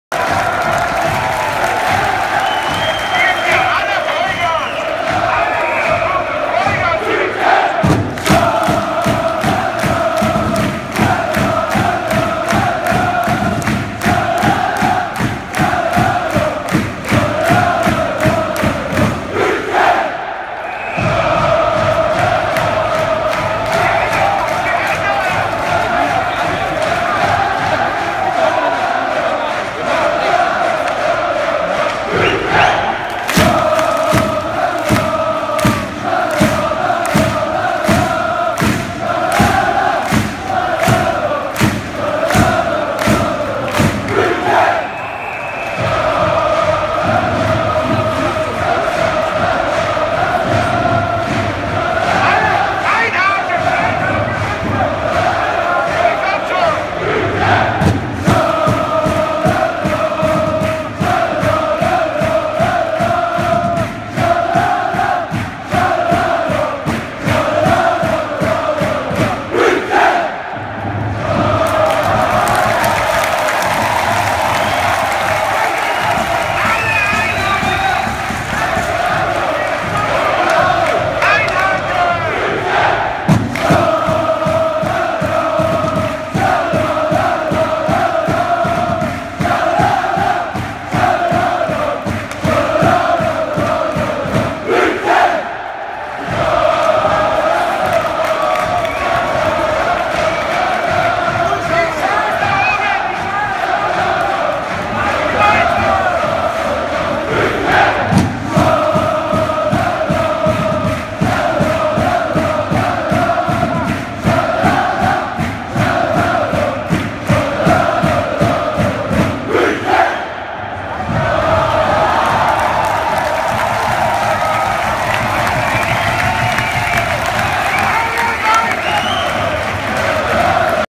Nord-Süd-Wechsel
EHC Redbull München Wechselgesang der beiden Kurven
1. Finale gegen Wolfsburg 2017